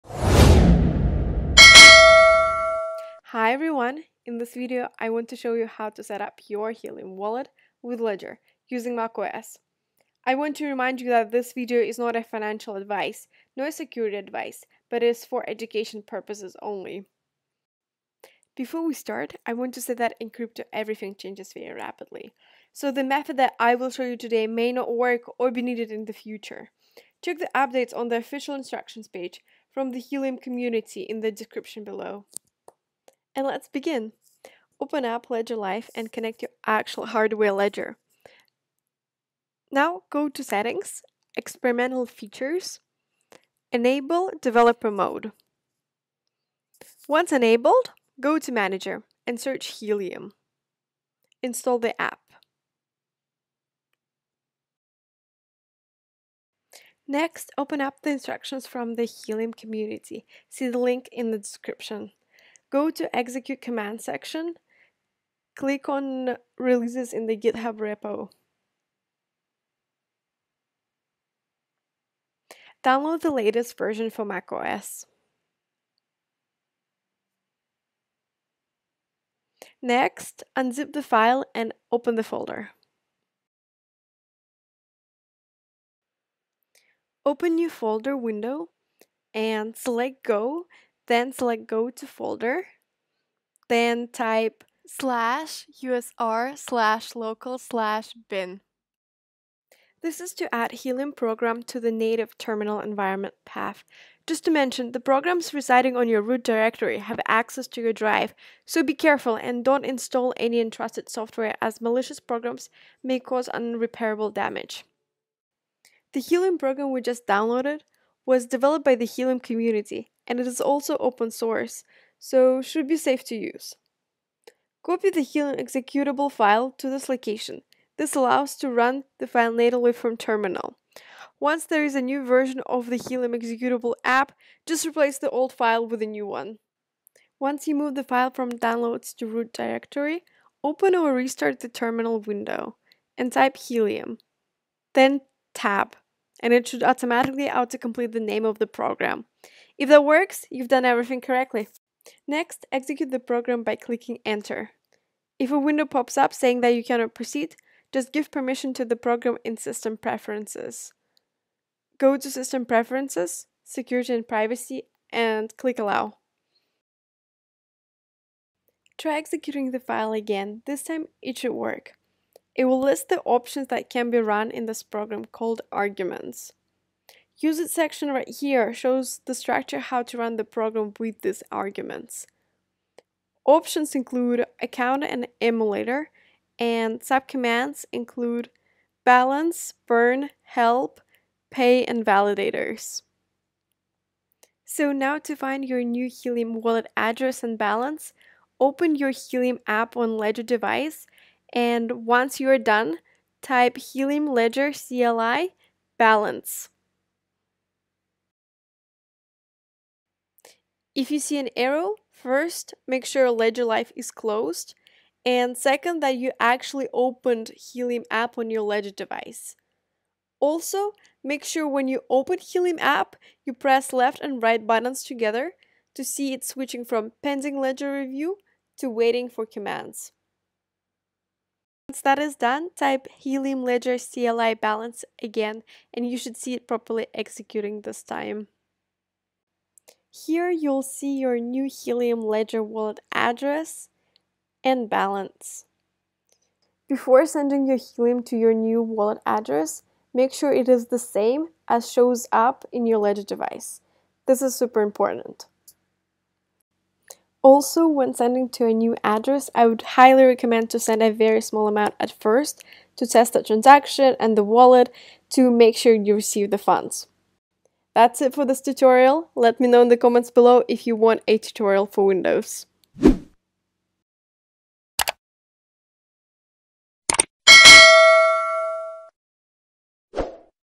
Set Up Your Helium Wallet with Ledger
In this tutorial, I show you how to set up your HELIUM wallet with Ledger (Helium Ledger CLI) using MacOS

Timecodes
0:00 Intro
0:33 Ledger Live
0:56 Downloading Helium CLI Program
1:26 Integrating with terminal
2:20 Terminal
3:14 Helium Wallet Address and Balance
4:27 Final remarks

If you see this on a command line "error: Error accessing Ledger HID Device. Be sure that Ledger Live is not running."
- close Ledger Live and open Helium App on your Ledger device (double click until you see "Waiting for Commands"